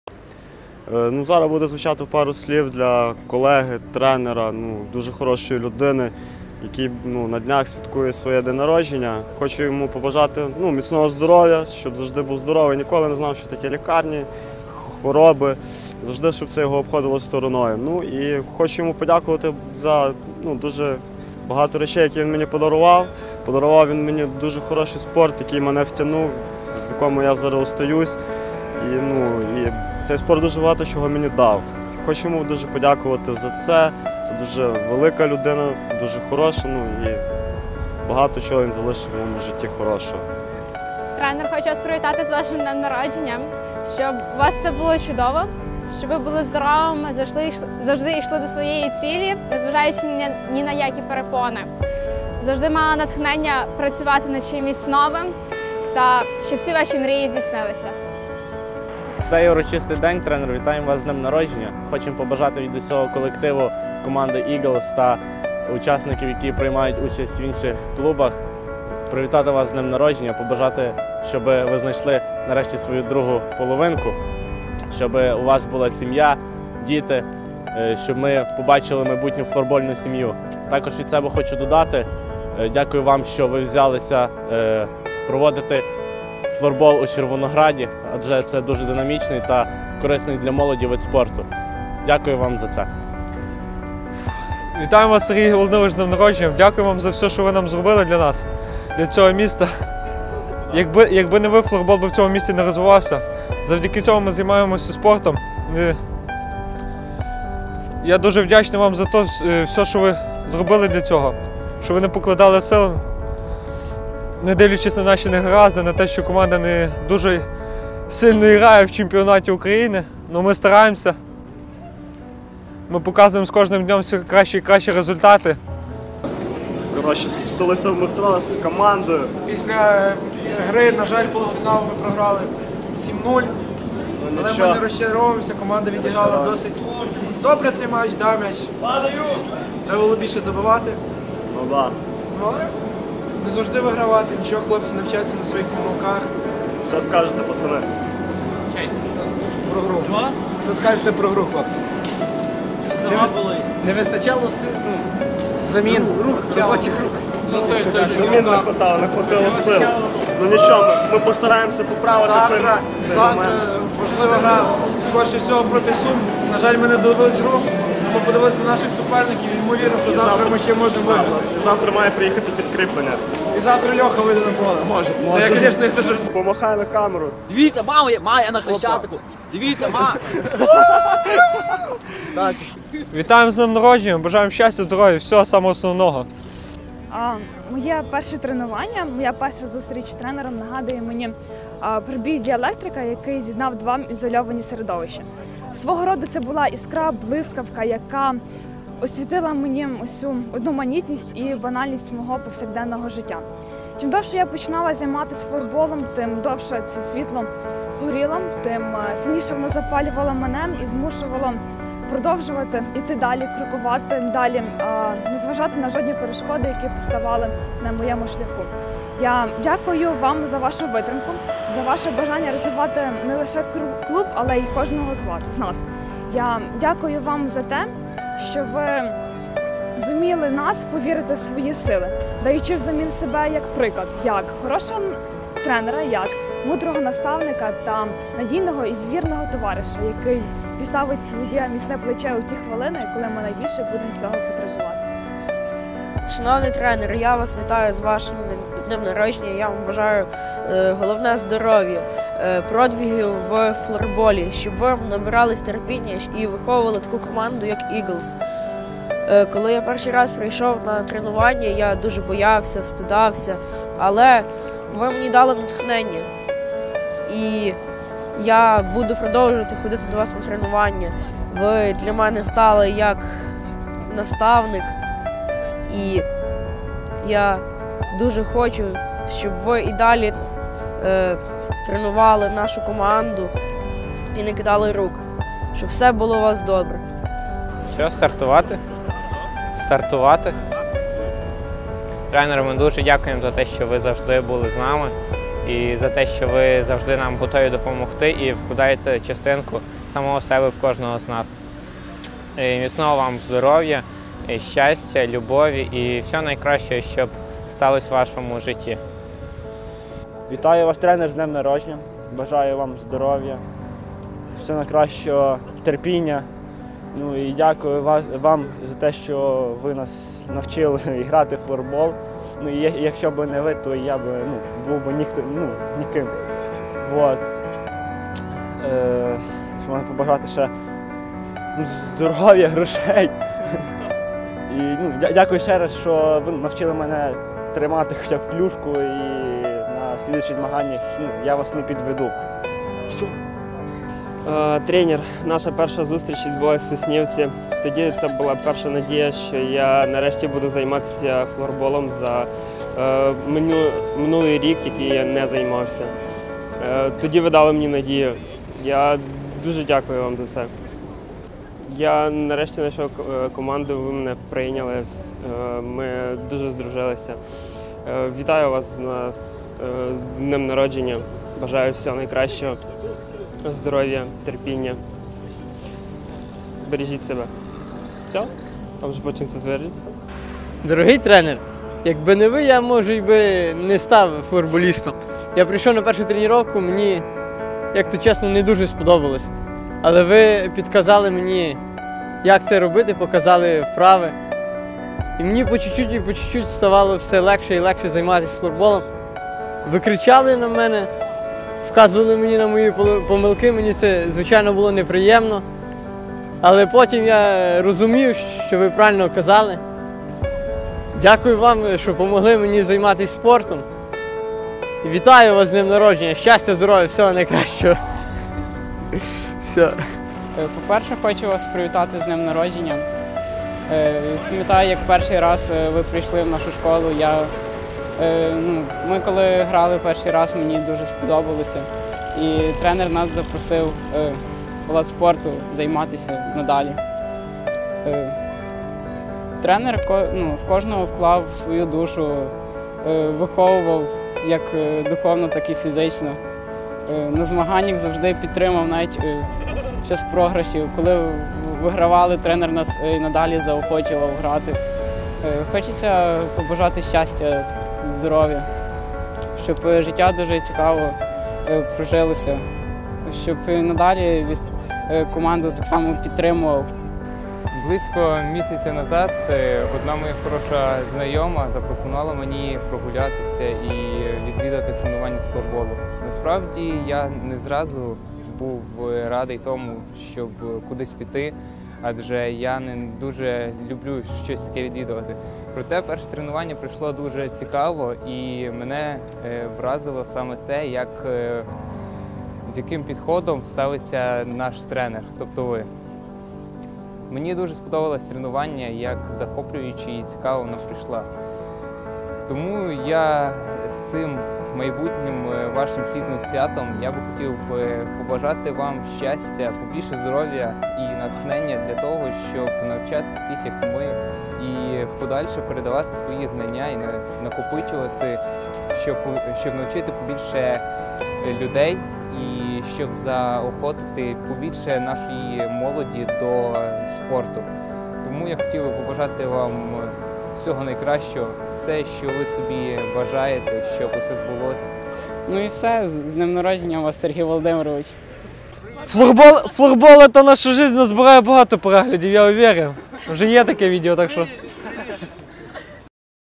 Happy birthday, Coach!
Сьогодні, 26.12. святкує своє день народження хороша людина, тренер та президент флорбольного клубу "Eagles" Chervonograd - Сергій Різник.
Свої слова привітань та подяки виразили гравці клубу Eagles.